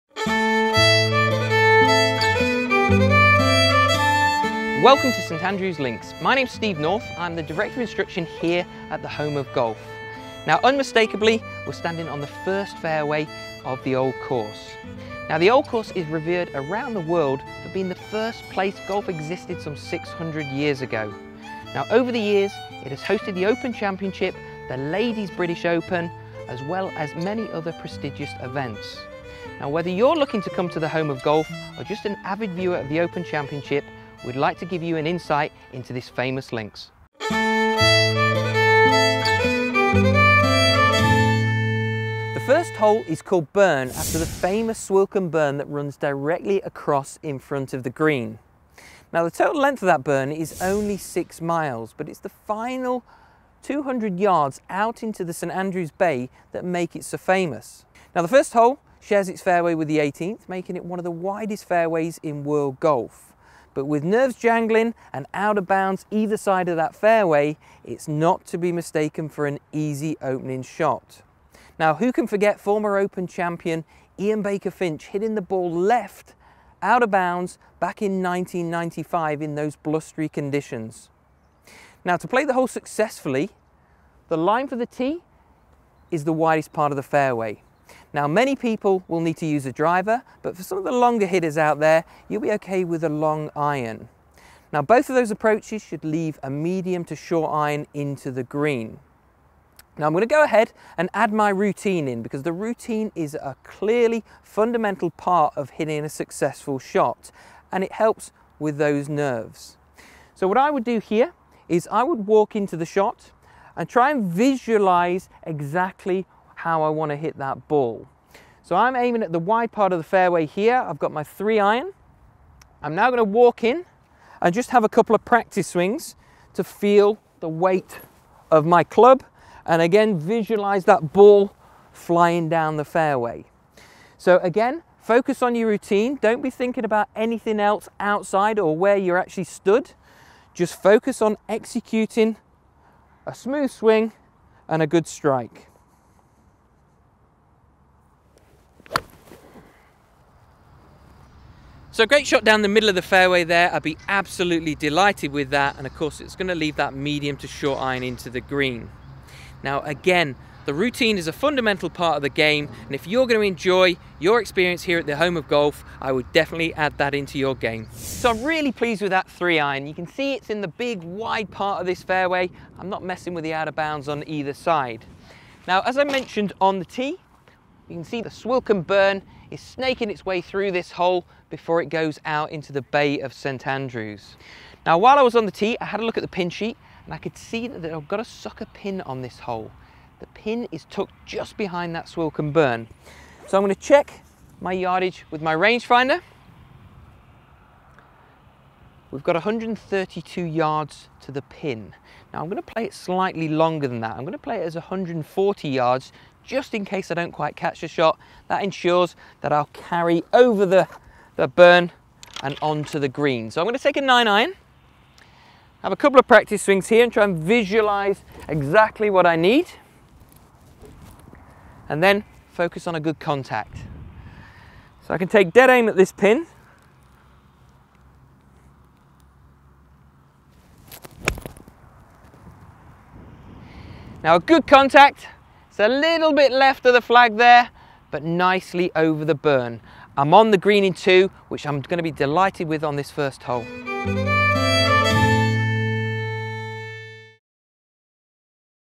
How to Play the Old Course with Steve North - Hole 1 - Burn
Join our Director of Instruction, Steve North for a unique insight into how to play the iconic Old Course here at the Home of Golf. In this first episode, Steve talks us through Hole 1 - Burn.